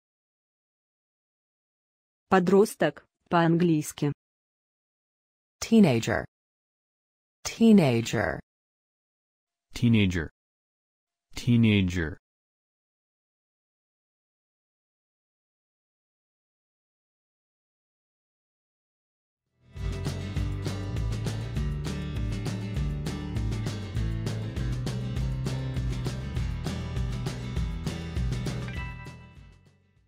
Подросток по-английски
Как правильно сказать Подросток по-английски? Слушай вместе с нами.
А как будет Подросток в переводе с русского на английский? Говори, как произносят американцы. И еще, как правильно написать слово Teenager?

Спасибо!